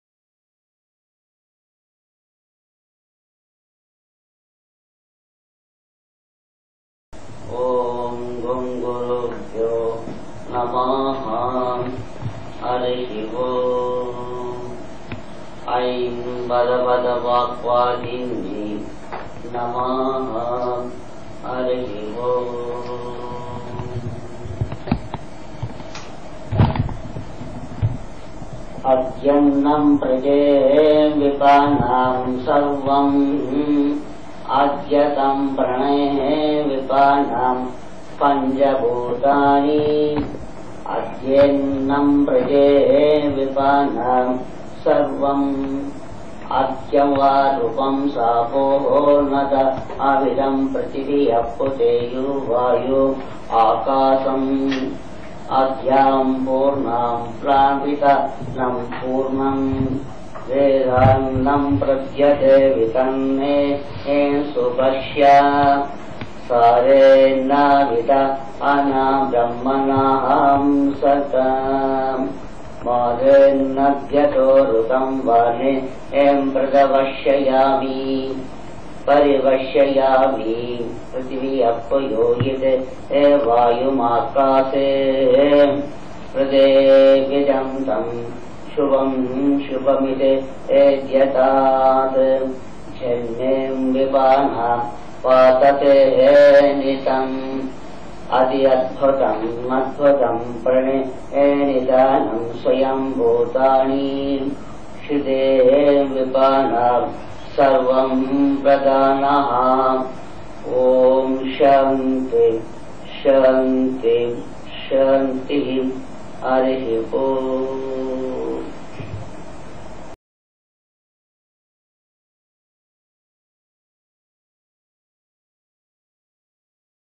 MANTRA CHANTING ARMY AT PUNE
H.H.Swamiji Given Lecture at Army Camp at Pune